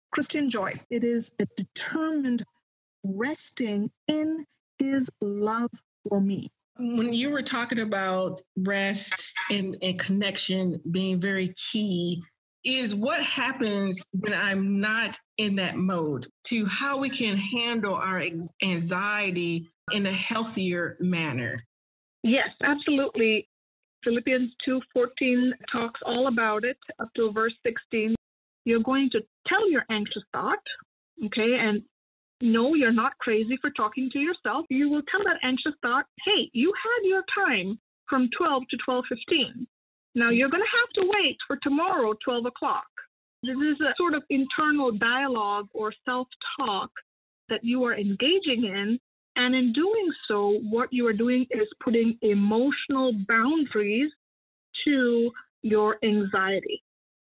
Spiritual Self-Care: E12 - Finding Joy in Adversity (Teaser)
New podcast episode identifies spiritual self-care methods that keep us finding joy. Learn how to overcome anxiety with God.